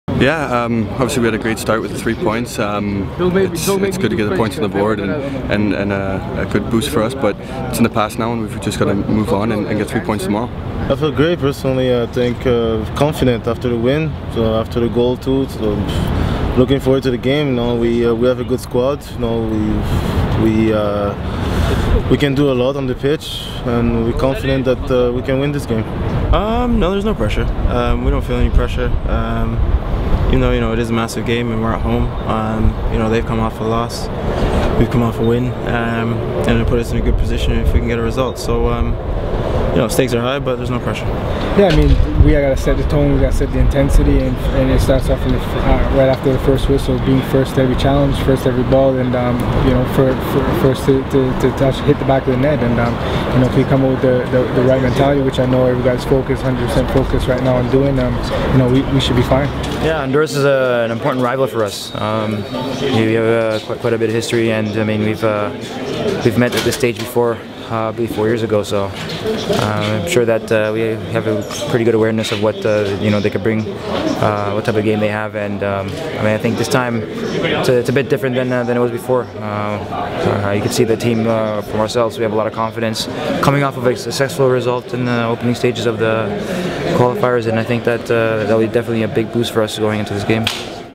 CANMNT: Players focused on Honduras qualifier
David Edgar, Olivier Occean, Simeon Jackson, Dwayne De Rosario and Julian De Guzman speak to the media ahead of Canada's FIFA World Cup Qualifiers match v Honduras on Tuesday June 12th at BMO Field.